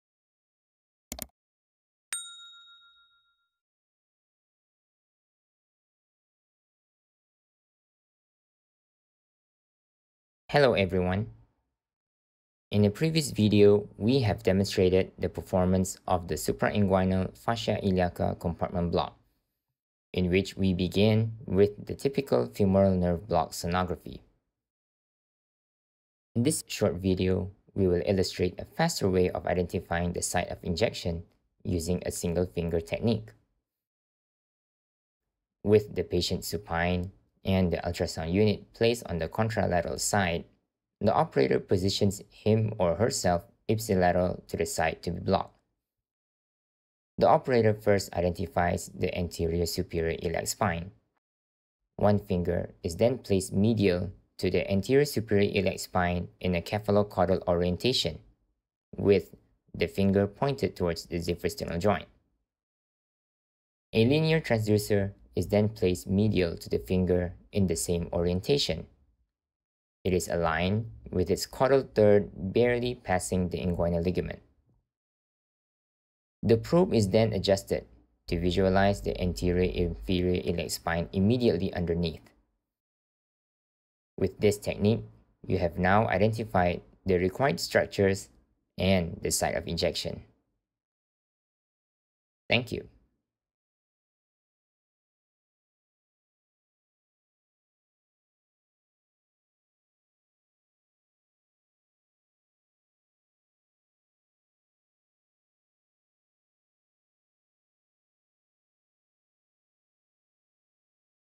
Shortcut Sonography for the Suprainguinal Fascia Iliaca Compartment Block (FICB)
This video demonstrates an alternative quicker method to perform the sonography of the suprainguinal fascia iliaca block. For those not familiar with this block, it is suggested that you watch our prior video on this technique

If you're interested in some of the behind the scenes work, specifically audio, check out this video on my personal channel